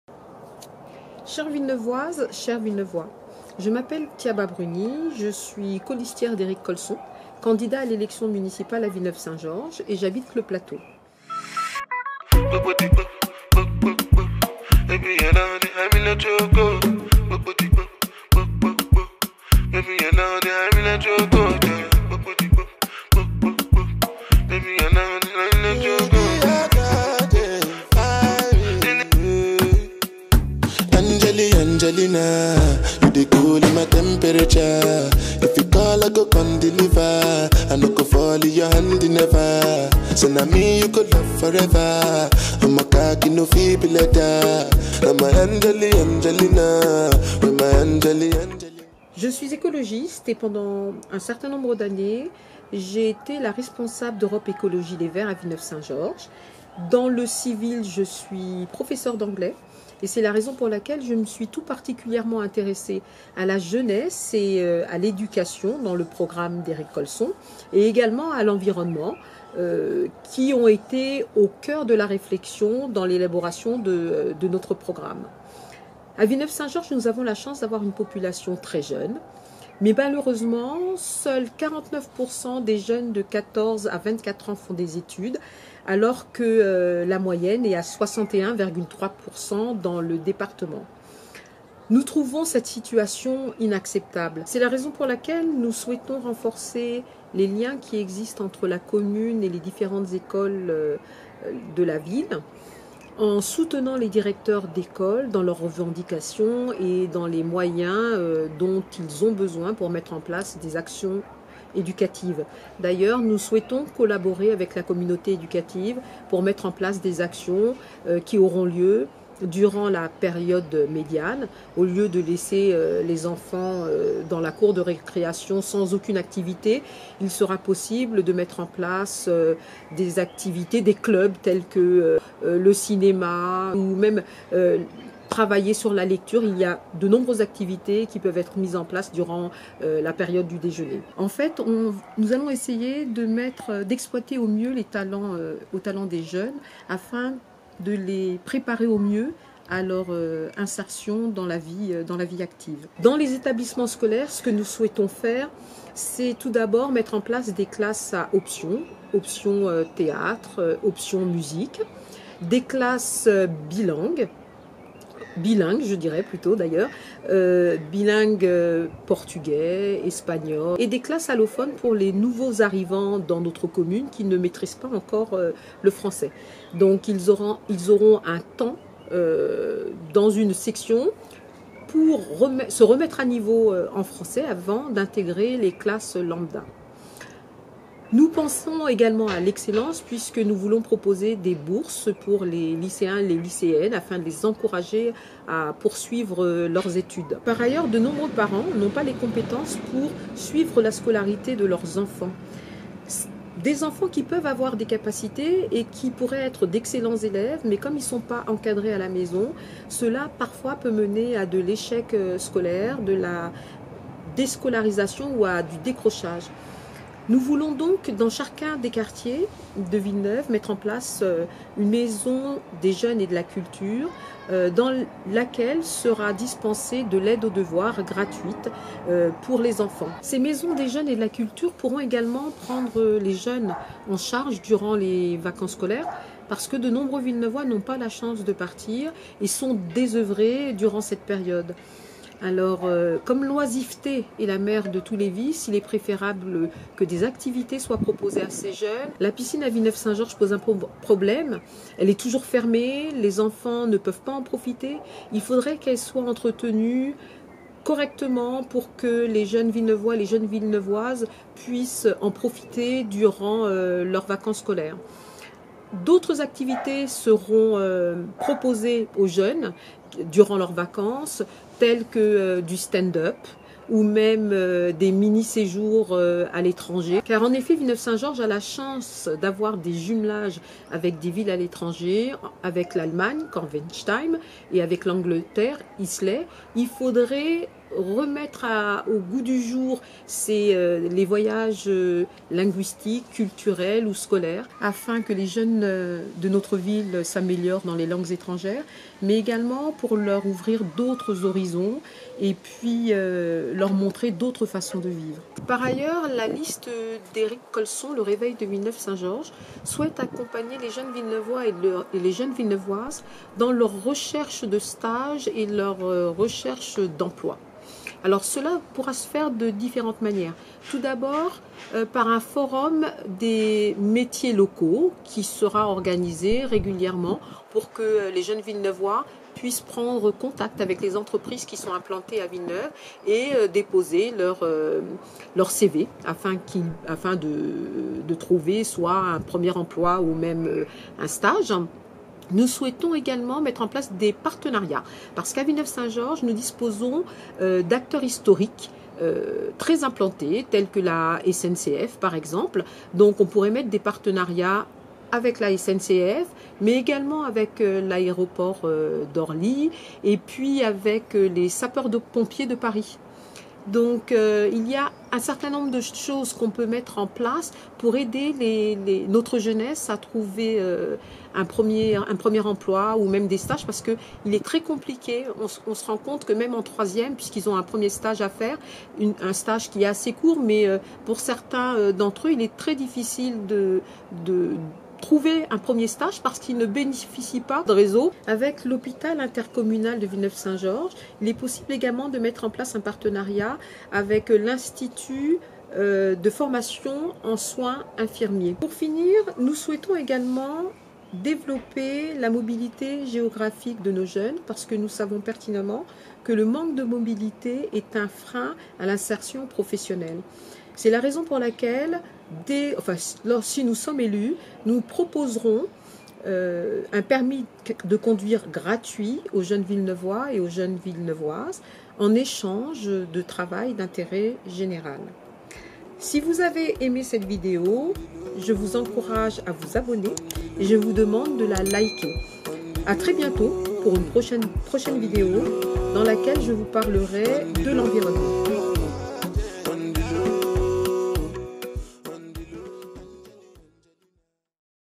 On en parle ! : L'Education à Villeneuve. Avec Thiaba Bruni !
Thiaba Bruni, seconde sur la liste d'Eric Colson, nous parle aujourd’hui des projets de la liste : Le Réveil de Villeneuve-Saint-Georges, Autour de L’éducation.